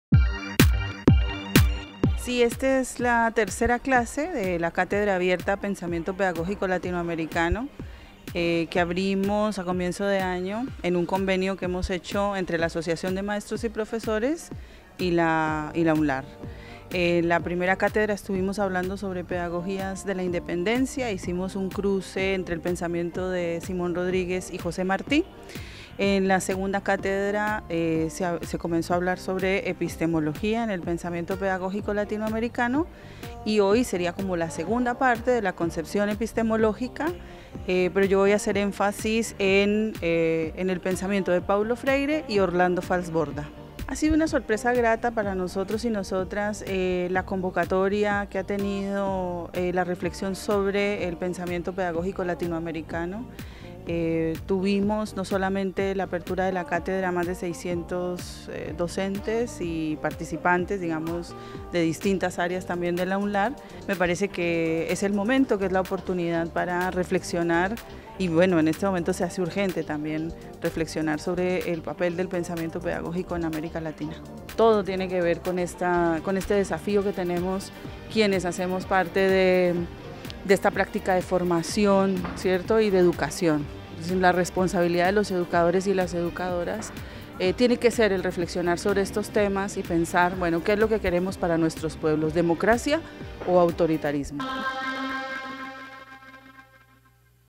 U Noticias - El papel del pensamiento pedagógico en América Latina como eje de formación a docentes
Se realizó el 3° Encuentro de la Cátedra Abierta de Pensamiento Pedagógico Latinoamericano, denominado "Epistemología de las Pedagogías Latinoamericanas: Paulo Freire - Orlando Fals Borda".